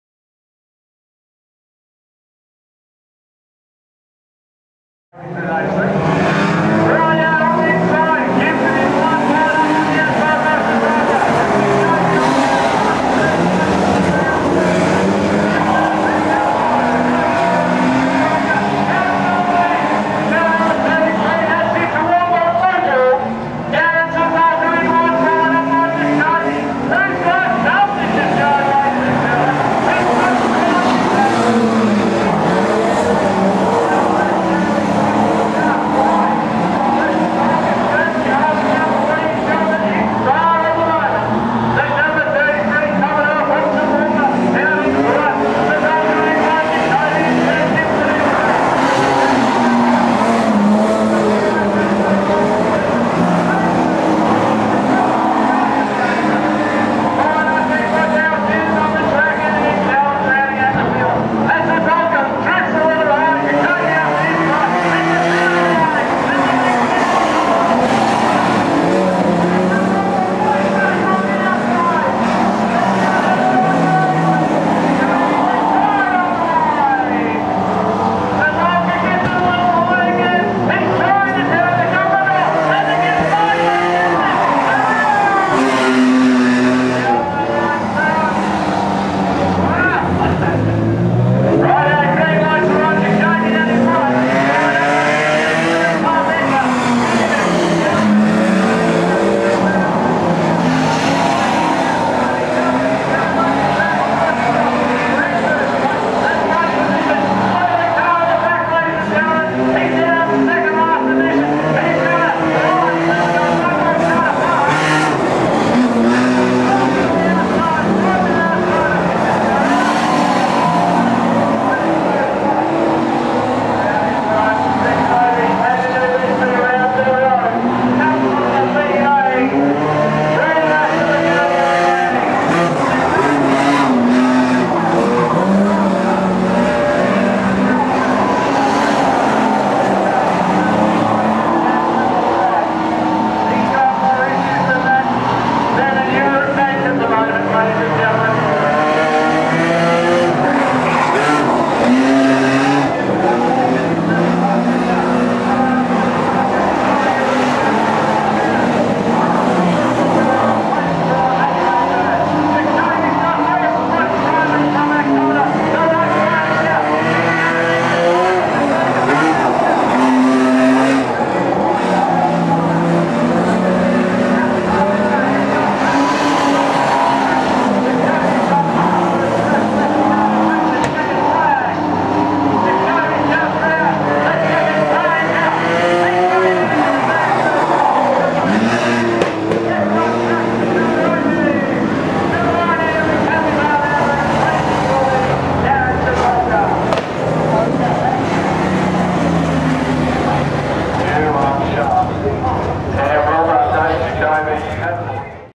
Modified Production - B-Main - South West Allstars Series (Round 02) - Charlton Raceway - 19.11.11
This is the B-Main for Round 02 of the Quantam South West Allstars Series held at Charlton Raceway on the 19.11.11